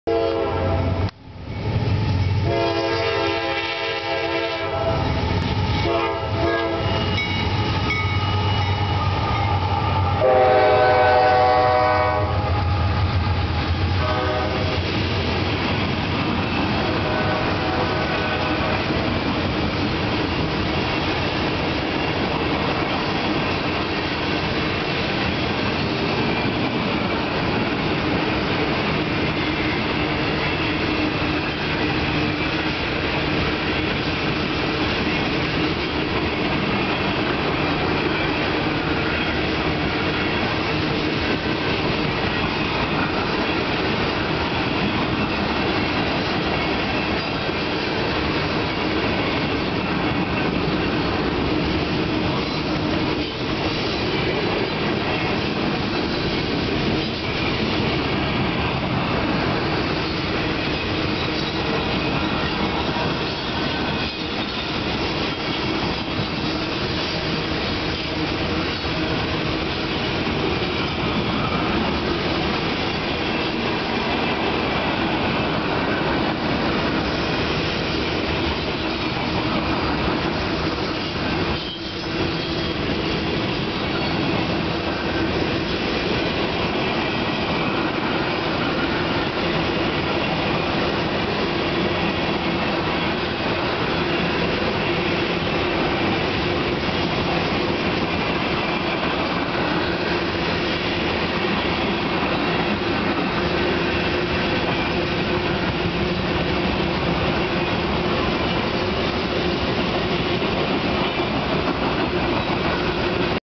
CSX @ Folkston 7/27/08 7:28 PM
Southbound coal headed thru Folkston.